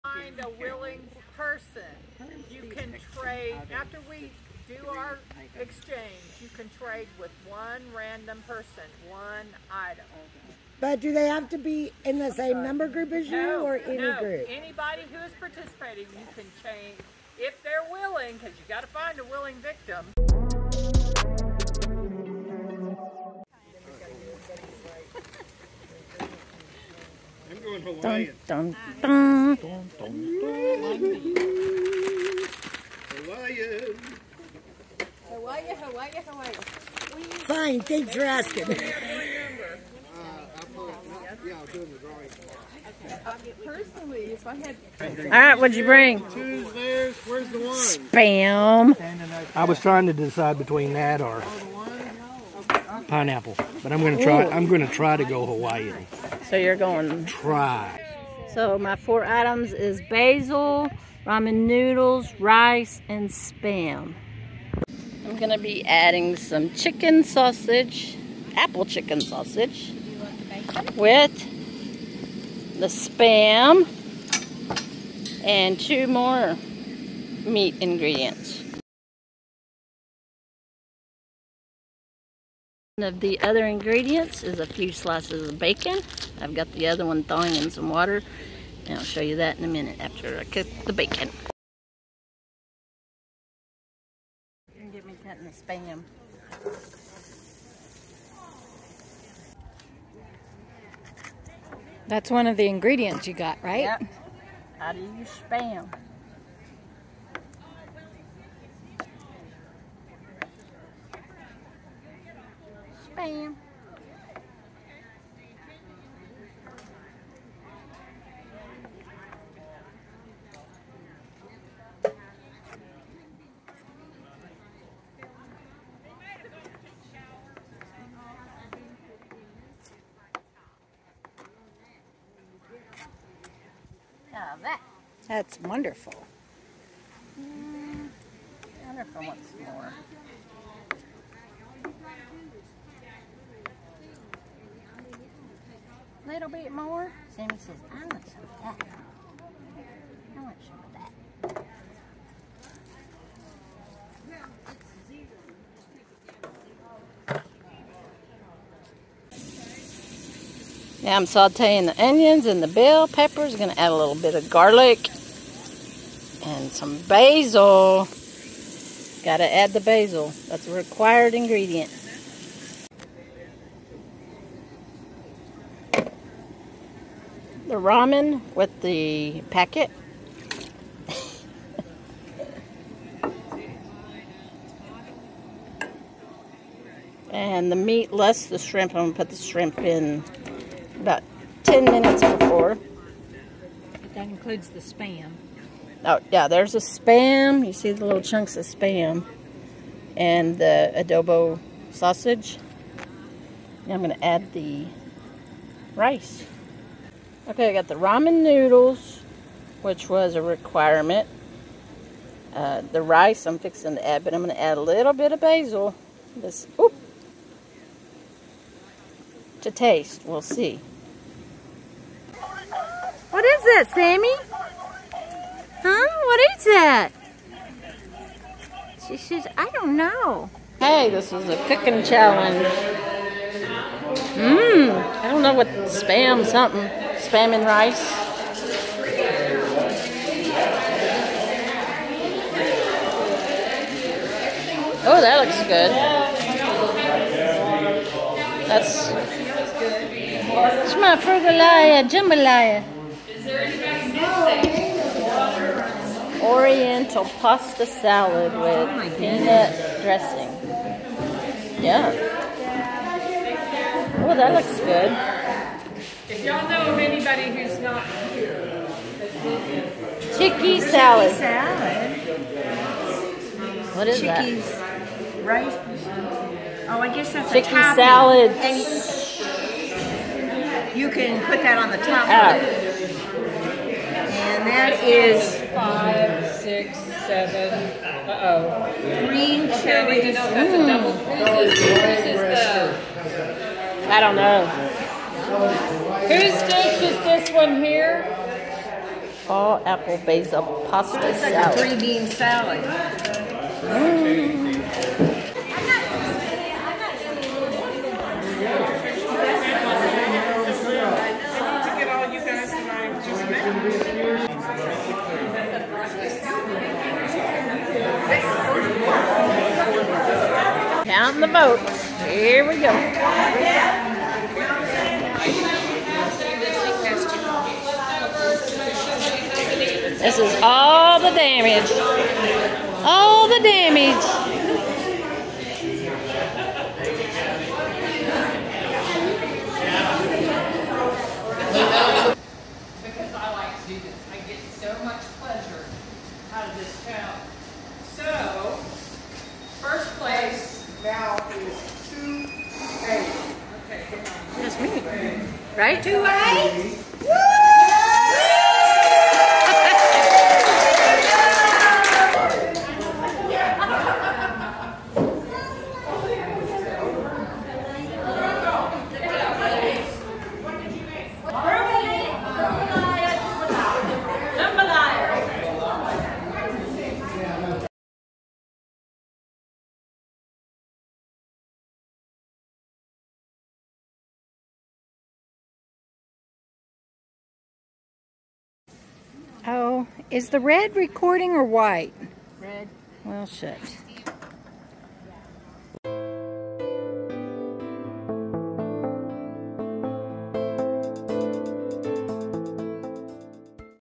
Lolapalooza 2022 - Gypsy Pair Cooking Challenge
Lolapalooza 2022 - Gypsy Pair Cooking Challenge.  I participated in the annual Gypsy Pair Cooking Challenge.  Watch the video to see if I was chopped or not.

CAMERA GEAR:
GoPro Hero 7
Pixel 6 Pro: